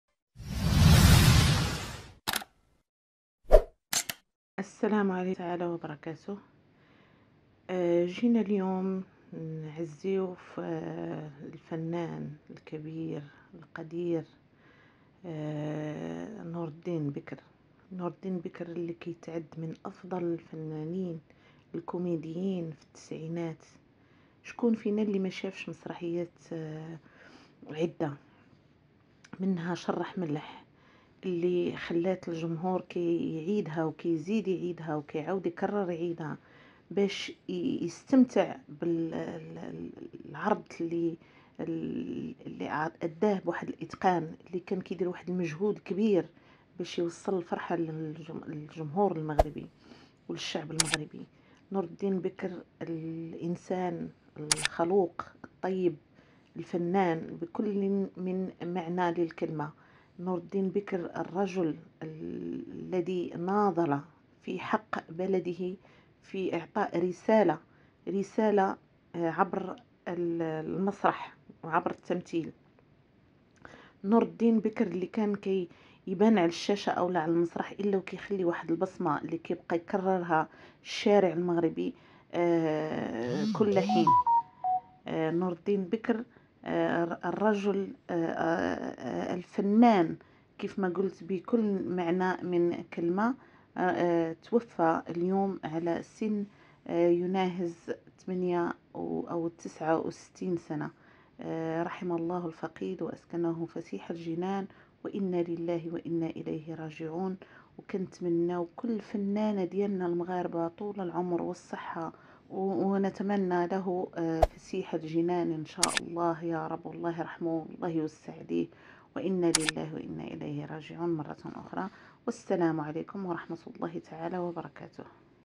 عاجل : وفاة الفنان نور الدين بكر إنا لله وإنا إليه راجعون/نور الدين بكر في ذمة الله
سبحان الله وبحمده سبحان الله العظيم اللهم صل وسلم على سيدنا محمد وعلى اله وصحبه وسلم تسليما...